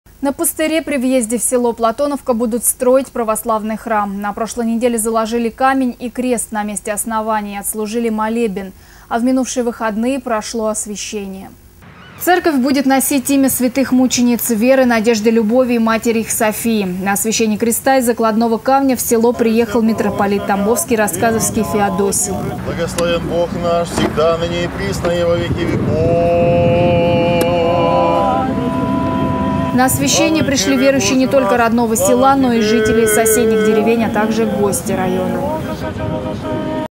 Камень и крест на месте будущей церкви в Платоновке освятил митрополит Тамбовский Феодосий
На пустыре при въезде в село Платоновка будут строить православный храм. На прошлой неделе заложили камень и крест на месте основания и отслужен молебен. В минувшие выходные прошло освящение.

Ирина Птуха